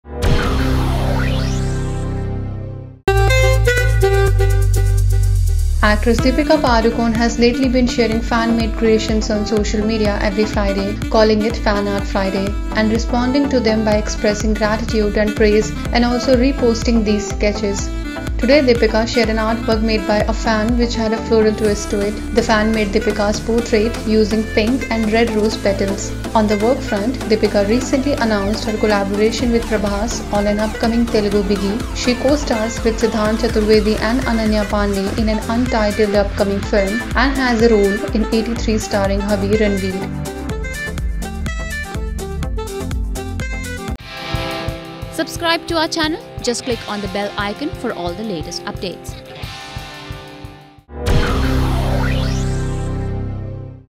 Deepika Padukone's 'FANart Friday'
Actress Deepika has lately been sharing fan-made creations on social media every Friday, calling it FANart Friday and responding to them by expressing gratitude and praise, and also re-posting these sketches.